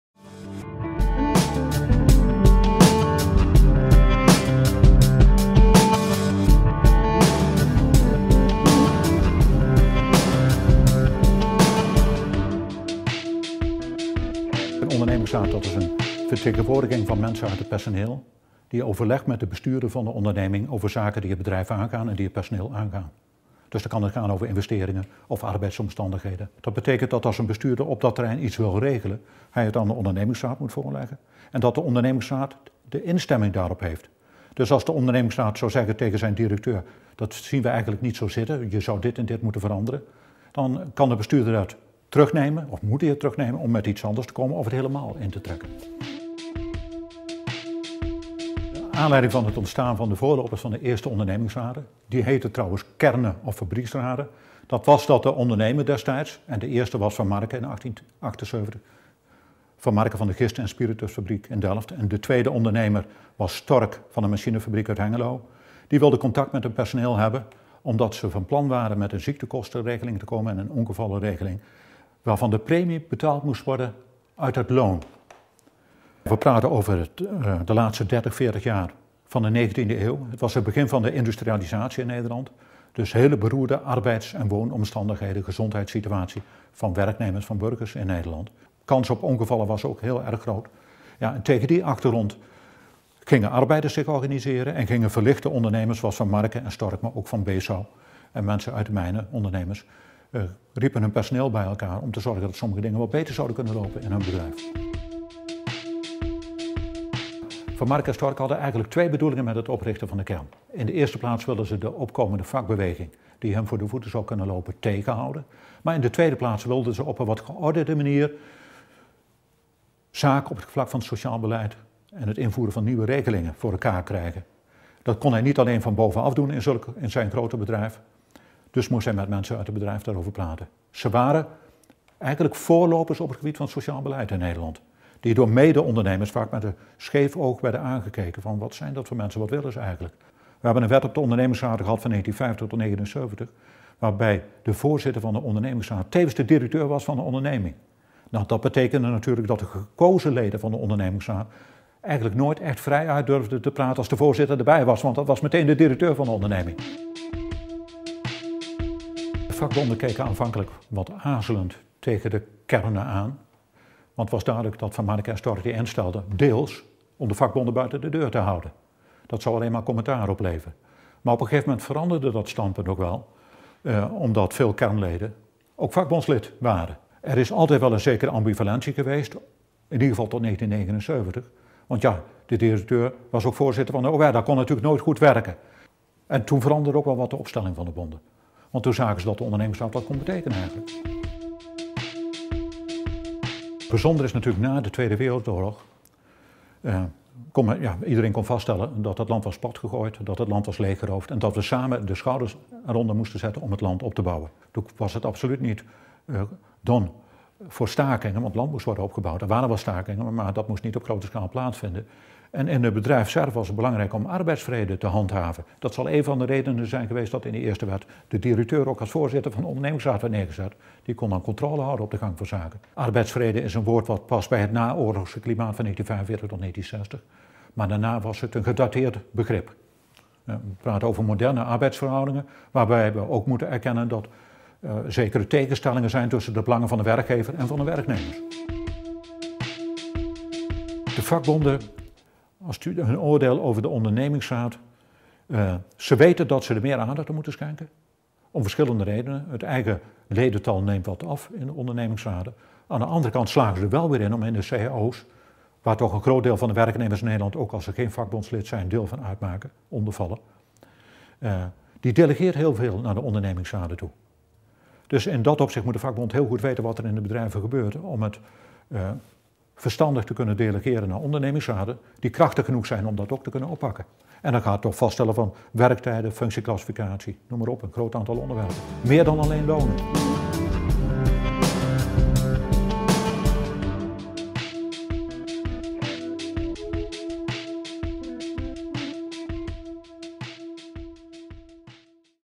VHV Leergang 2015 - College 3 - Ontstaan van medezeggeschap en ondernemingsraad
College van Harry Peer, oud-trainer en consultant ondernemingsraden, gehouden op 7 maart 2015 in De Burcht van Berlage te Amsterdam